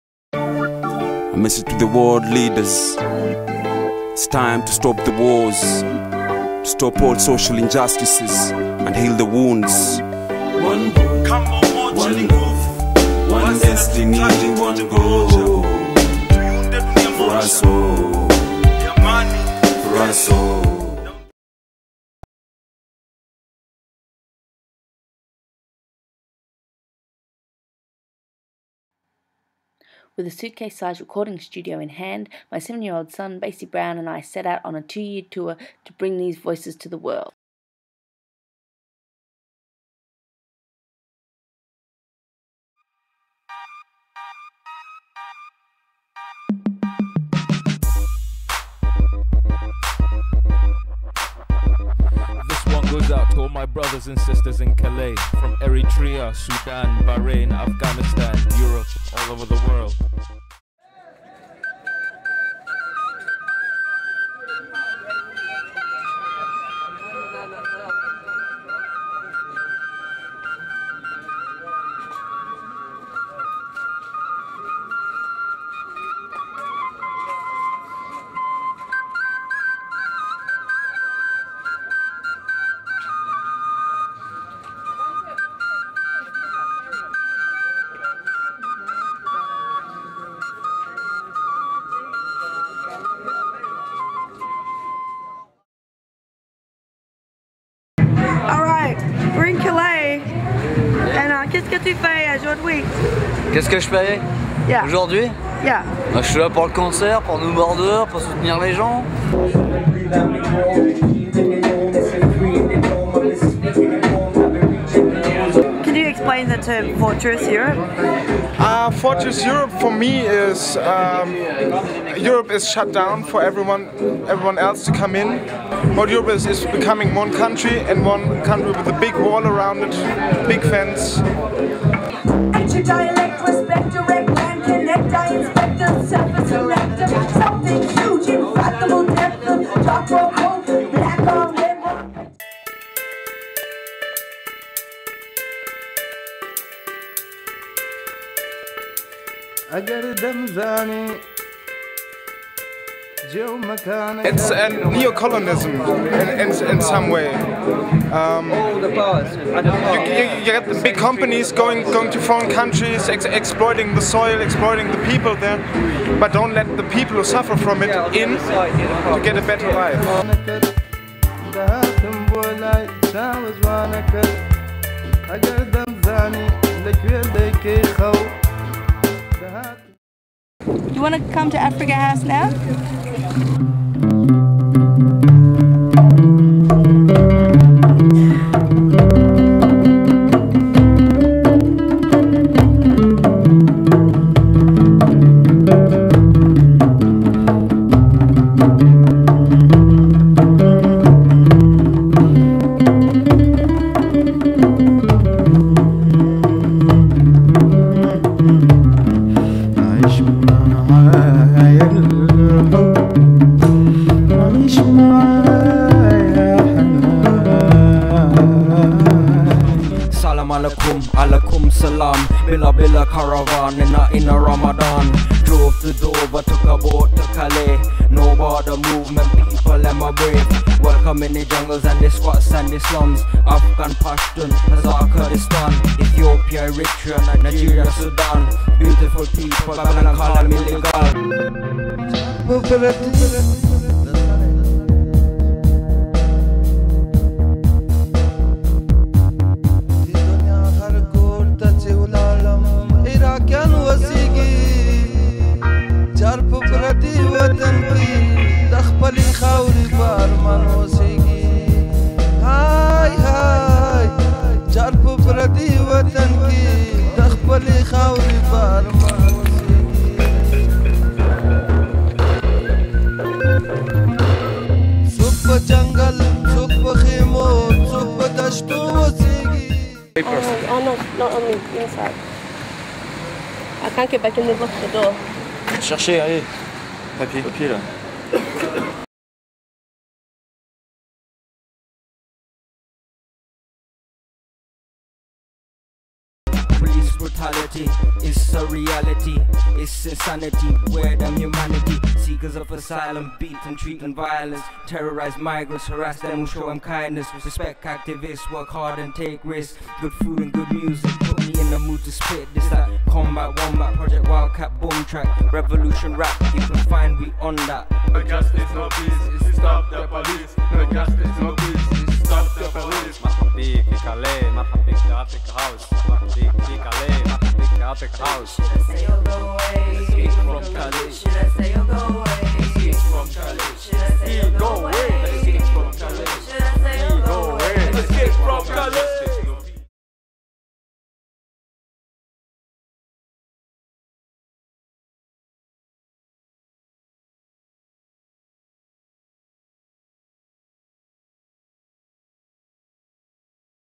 United Struggle Project part 1
This is the first part of the 6 part story of United StrugglesProject
So the recordings started in Calais France where refugees and migrants from many war-torn places are hiding in squats and camps on the run from constant  police harassment. We recorded with Afghani, Kurdish, Sudanese, Nigerian/UK, Indian/UK, Chadian and Eritrean.
Monkeymarc, Fran and I joined forces with No Border collective to put on the festival 'Halfi de Bla Hudud' (music with out borders)
During the recordings we got evicted, chased, equipment broken and arrested. I was sent to a detention center to be deported to Australia without my child. It was hectic to say the least. Luckily for me No Border crew got my passport to the authorities in time. But unfortunately for many of the people I met, escape from Calais was not that easy .. the difference is in a piece of paper..  The injustice and racism I witnessed from the government police and the local community towards the migrants and towards groups working in solidarity with them was extreme. It reinforced my commitment to braking those borders with music.
Im still wondering when 'making music' officially became illegal in France...